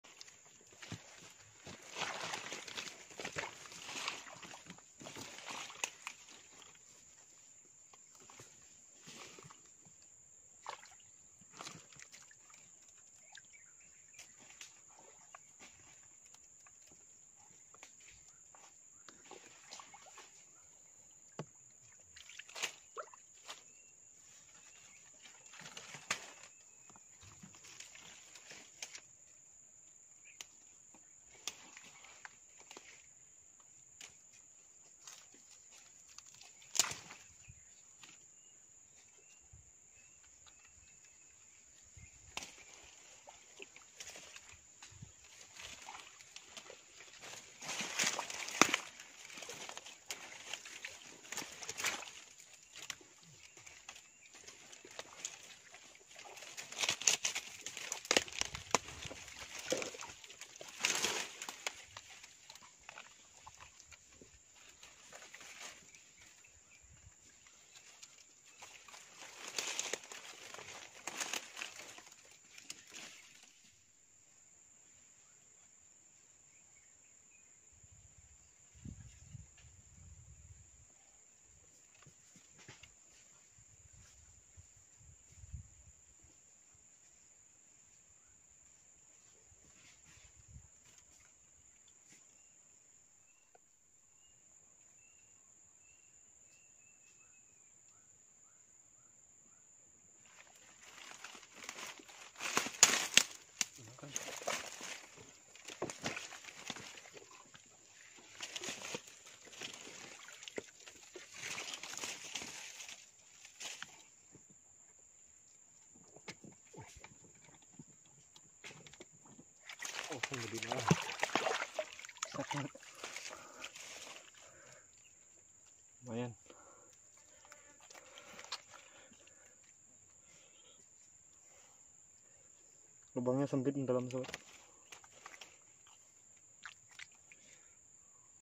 Amazing Fishermen Used Technic To Hook Catfish
Welcome to Fishing Bloggers . We are please to show you how to fish in countryside with traditional tools and custorm and some creating tools. Let's enjoy and have fun.